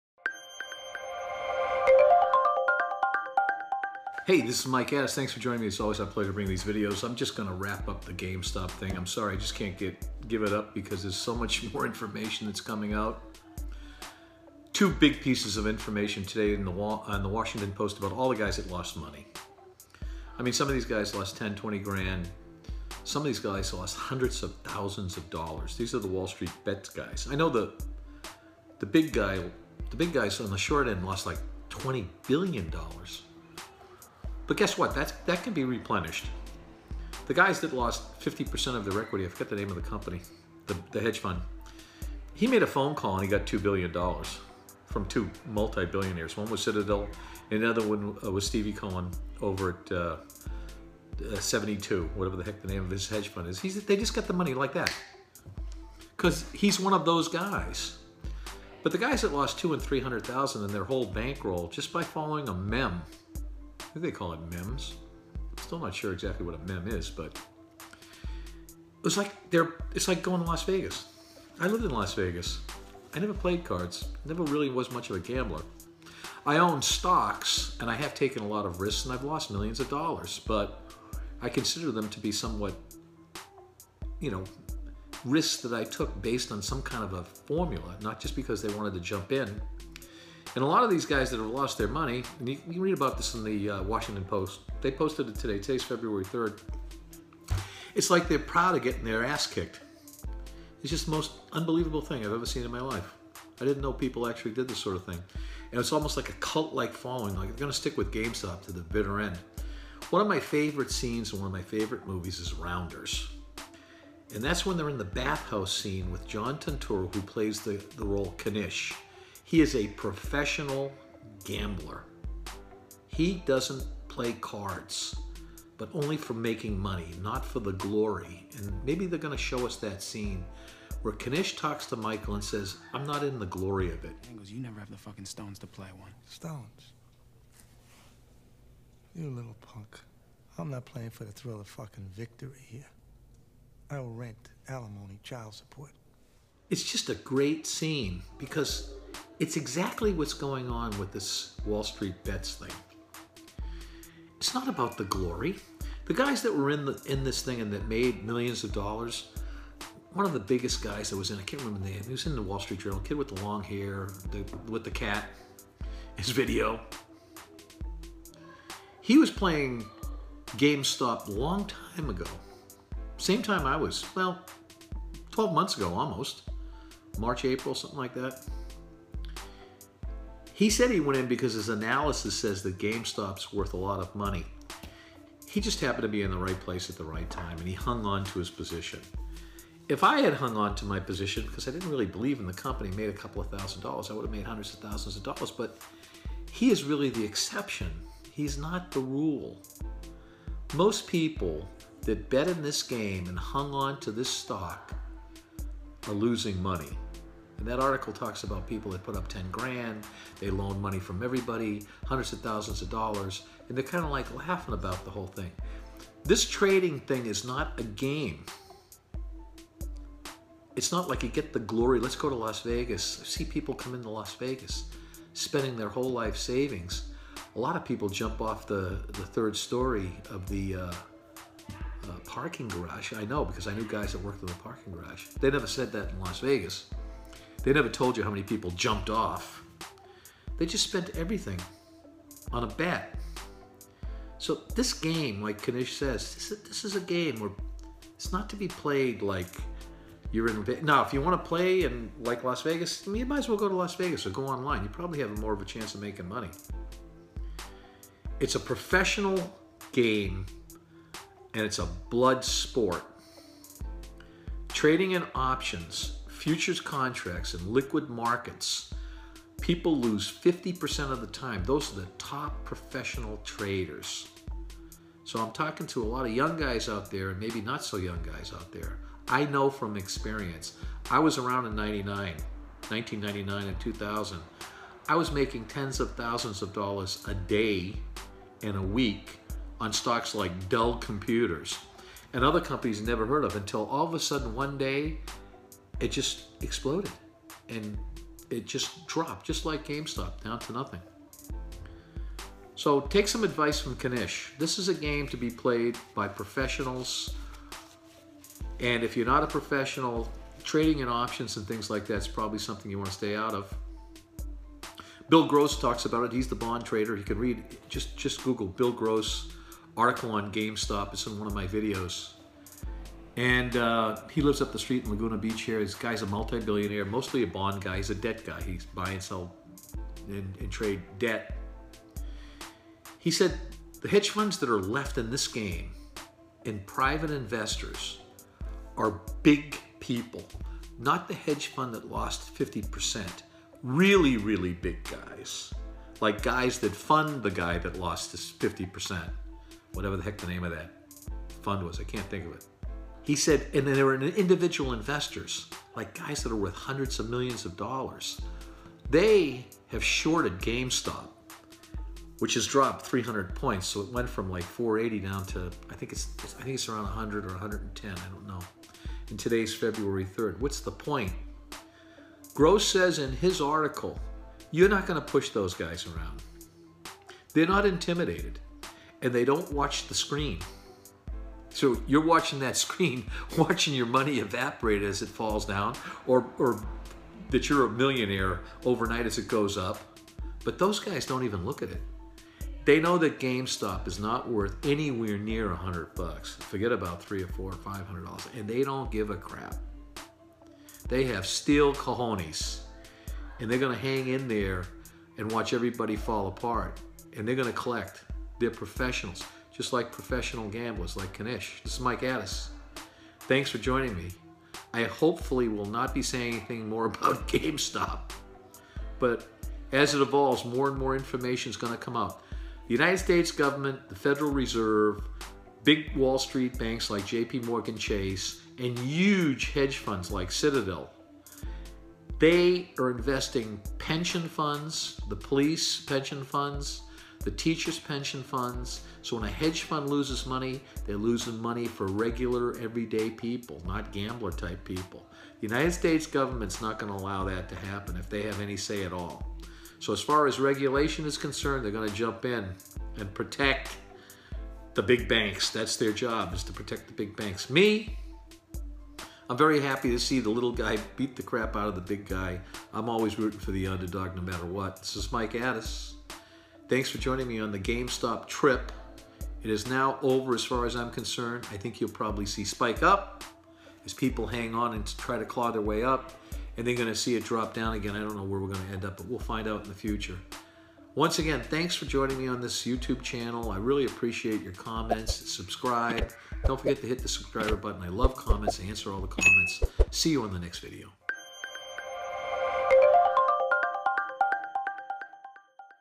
GameStop, Blood Sport, Or Gambling Video | We look at the fast rise then crash of GS and why?
Hello Viewers!
Welcome to another GameStop video!!
GameStop, blood sport, or gambling video. We look at the fast rise then crash of GS and why? GameStop's run-up to over $500 then crash down to earth now around $75 is a classic case of how markets can be moved and manipulated. Usually by large Hedge Funds but now by crowds who banded together. As I predicted, and others too, that GS would crash and go back to a true valuation. The reason for the crash is bigger Hedge Funds and huge investors placed more short bets and the result is here. In this video, we will see what billionaire investor Bill Gross says as to why the inevitable crash happens. And why option trading sometimes works but more often fails. Its gambling? Or blood sport?
Mike Addis

👉 Instagram: https Hope you guys enjoy this!